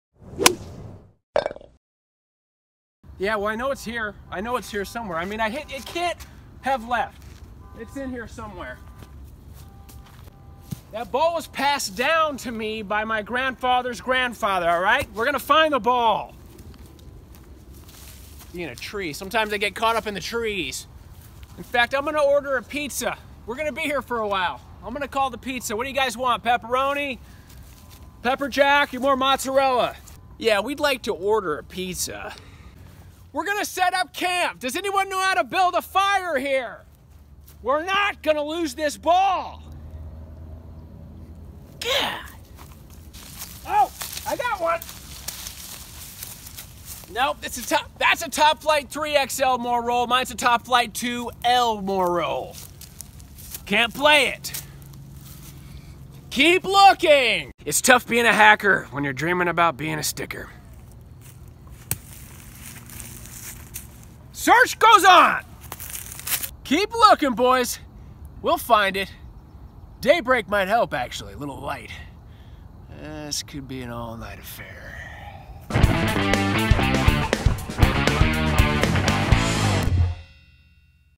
Stick and Hack Short: Search For Ball Guy
The toughest part of playing golf? Not playing golf because you lost the ball. Anyone want to help Member #364 Chris Grounds look? We think he's still out there...